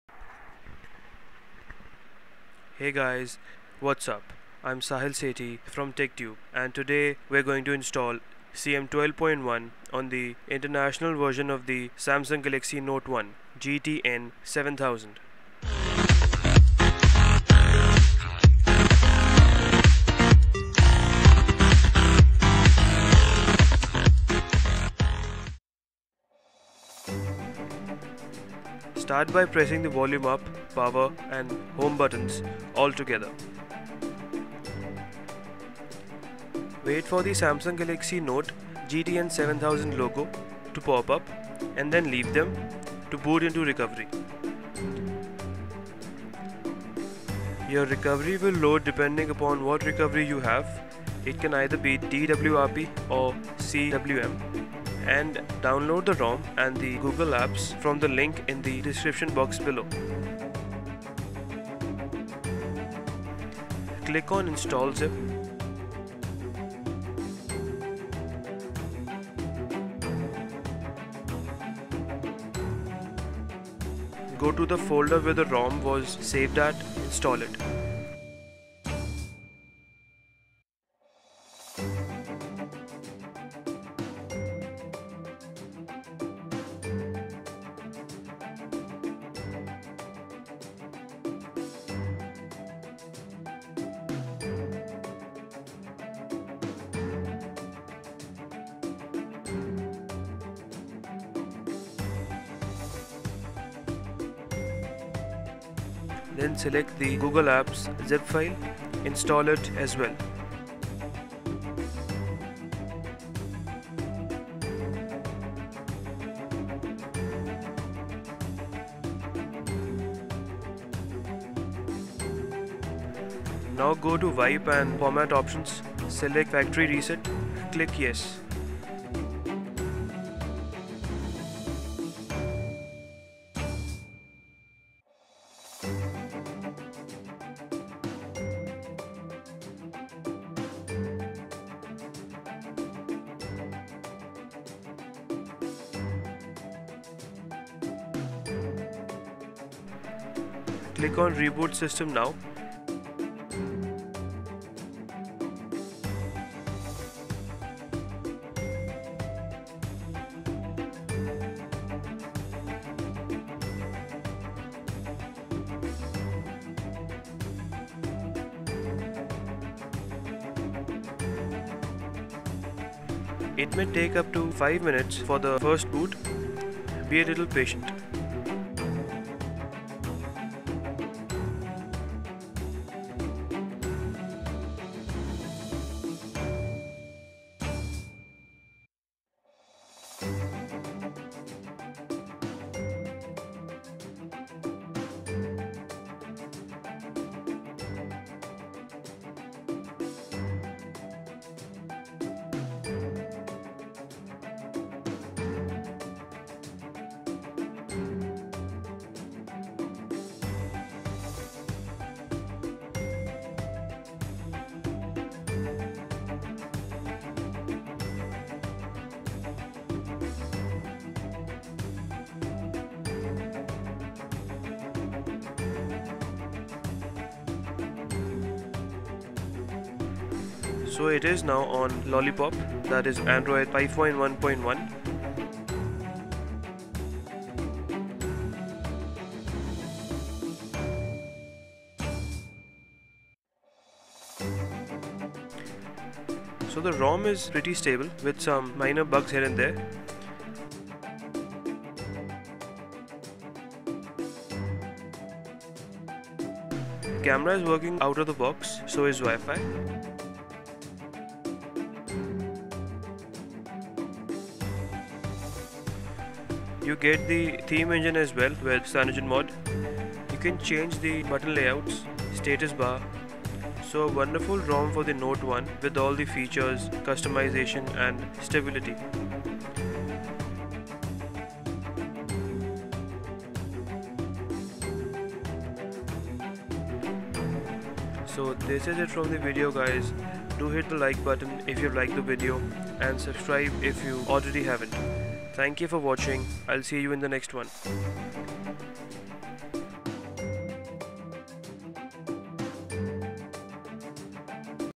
How to install Android 5.1 Lollipop (CyanogenMod 12) on Samsung Galaxy Note 1(N7000)
Here's How to install Android 5.1 Lollipop (CM12 ROM) on the Galaxy Note (N7000) by Tech Tube.